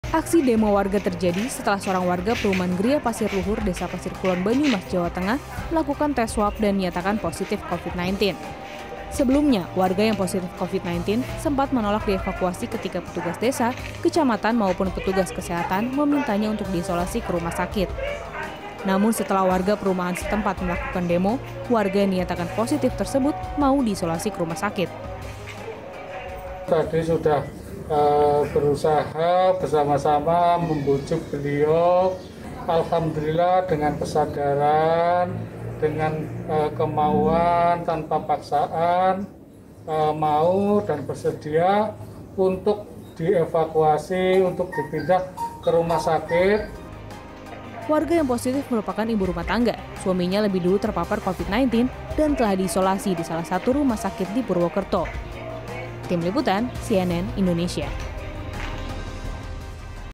Warga Protes Minta Pasien Covid-19 Dievakuasi ke RS
Aksi demo warga terjadi, setelah seorang warga perumahan Griya Pasir Luhur, Desa Pasir Kulon, Banyumas, Jawa Tengah, melakukan tes swab dan dinyatakan positif covid-19. Sebelumnya, warga yang positif covid, sempat menolak dievakuasi ketika petugas desa, kecamatan maupun petugas kesehatan memintanya untuk diisolasi ke rumah sakit.

Namun setelah warga perumahan setempat melakukan demo, warga yang dinyatakan positif tersebut, mau diisolasi ke rumah sakit.

Like & Follow Facebook:
CNN Indonesia

cnnindonesiatv